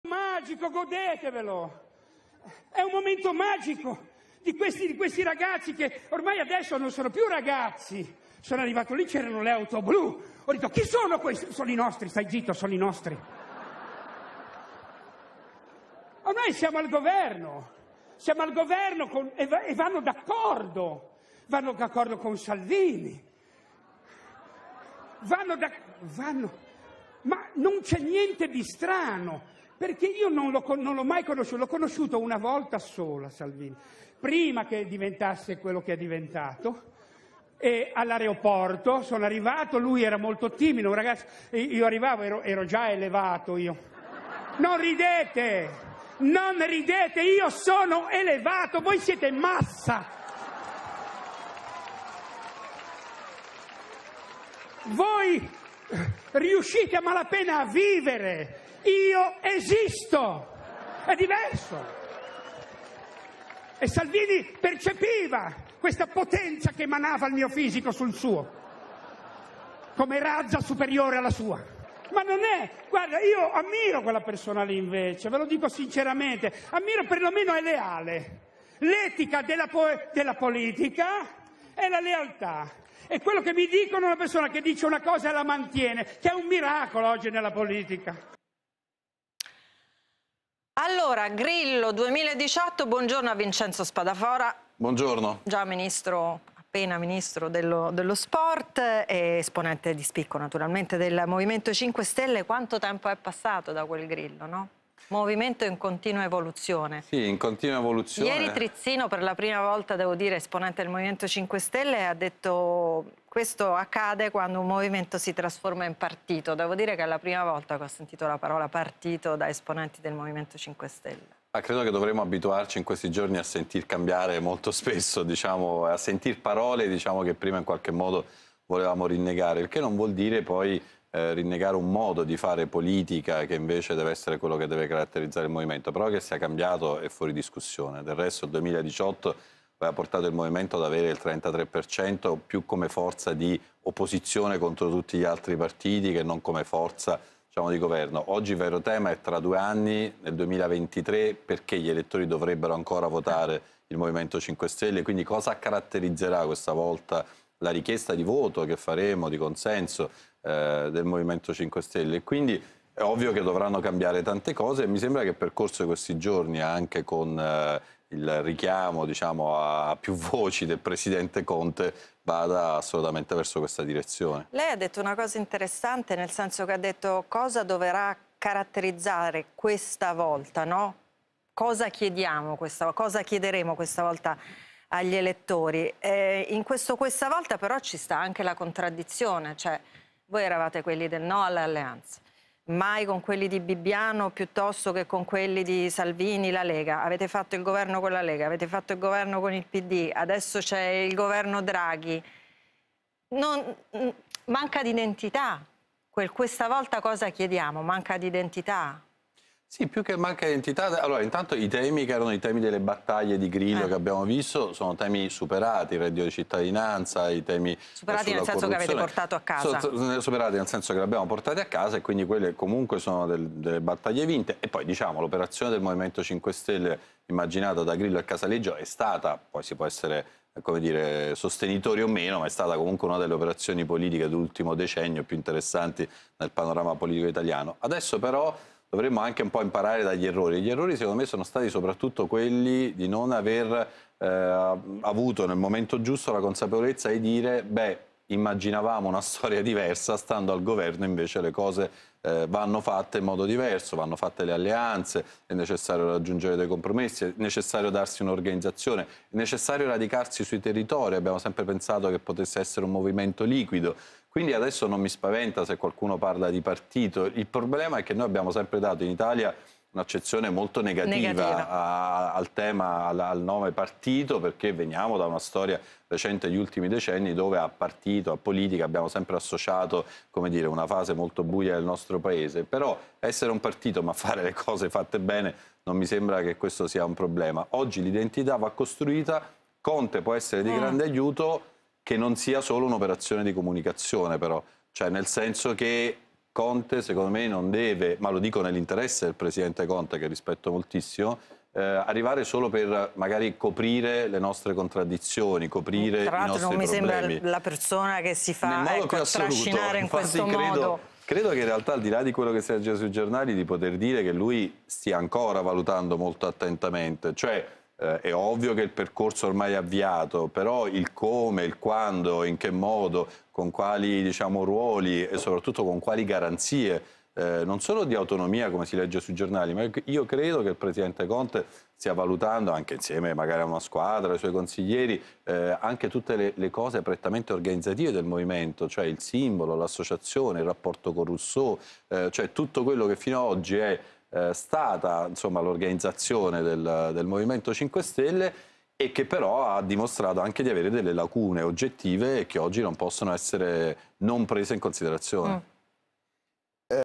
Conte in MoVimento, Vincenzo Spadafora: "Non deve arrivare solo per coprire i nostri problemi, ...
L'intervista all'ex Ministro Vincenzo Spadafora: "Il M5S è cambiato, stando al governo le cose vanno fatte in modo diverso. Conte non deve arrivare solo per coprire i nostri problemi, sta ancora valutando"